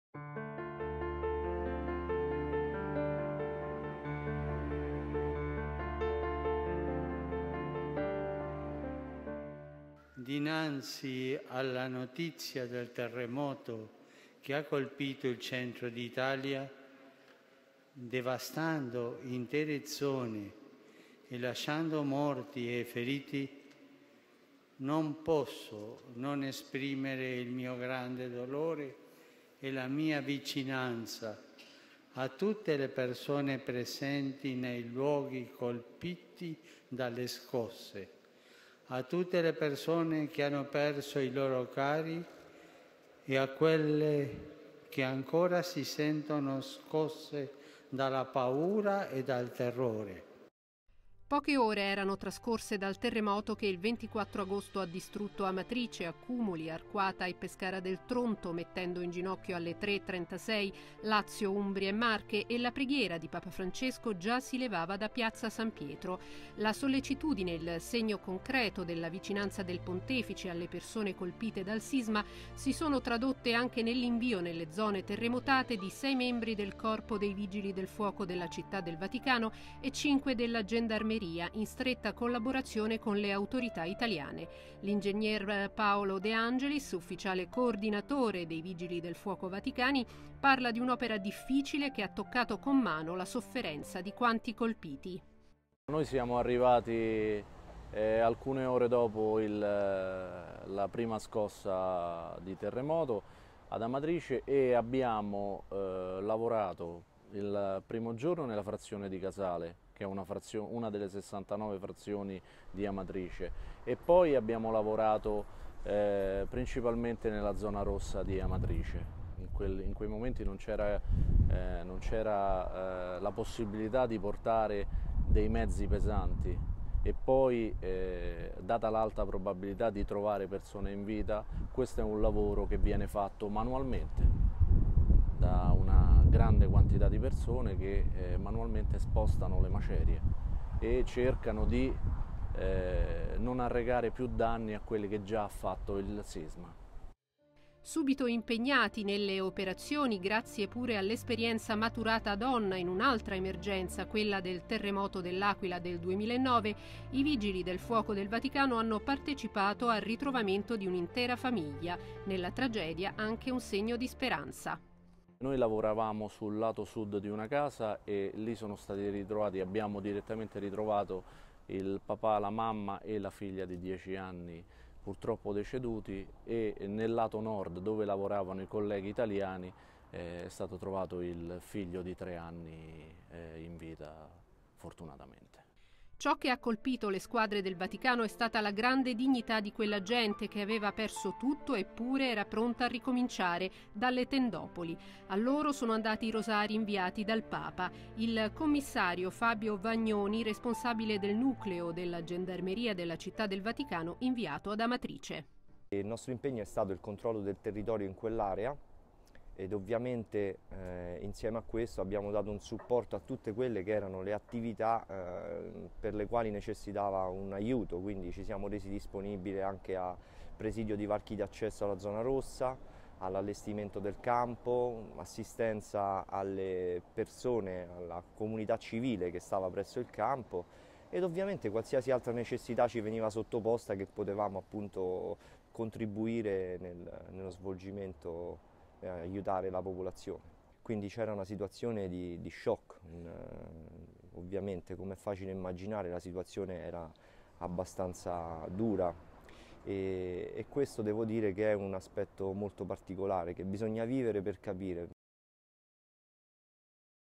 Vatican Magazine "Mani che spostano macerie" 09.09.2016
Innumerevoli le manifestazioni di vicinanza a quanti sono stati colpiti dal sisma in Centro Italia. Con noi l’ingegner Paolo De Angelis, ufficiale coordinatore dei Vigili del Fuoco vaticani e il gendarme Fabio Vagnoni.